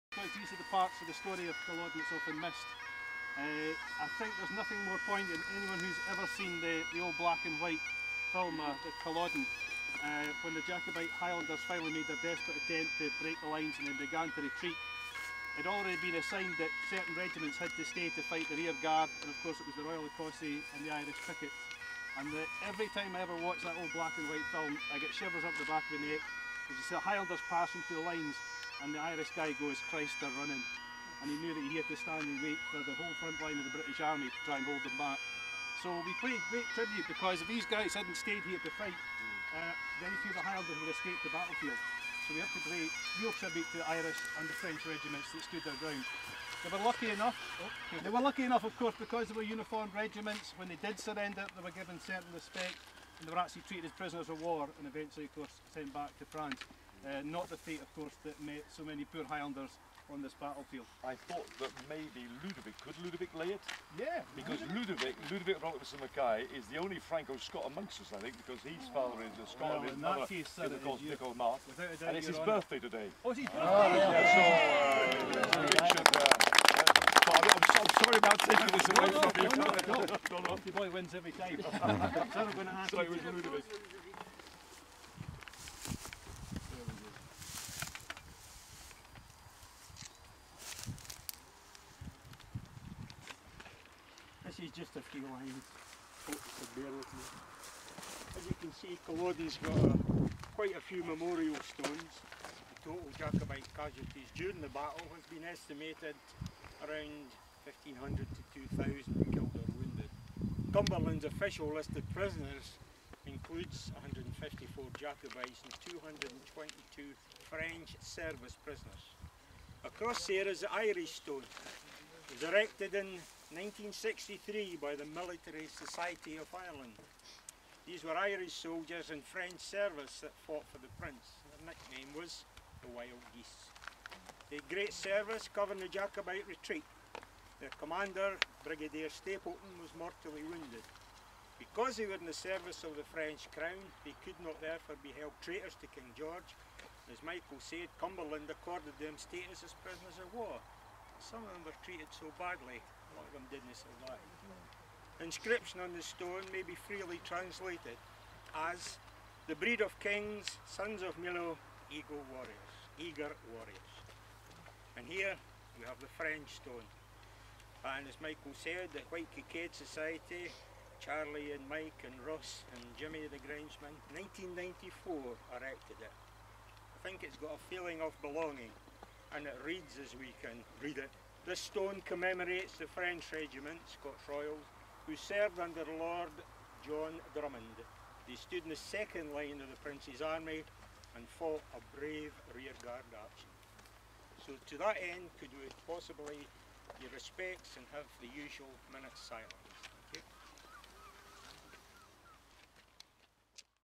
Battle of Culloden Anniversary French Regiment Royal Scots Wreath Ceremony
The memorial stone was erected in 1994, to commemorate the part the Royal Ecossais played at Culloden. Commanded by Lord John Drummond, they were asked to form the rear guard after the defeat on Culloden Moor. Being uniformed and part of French forces they were given more protection from the government retribution than the highlanders.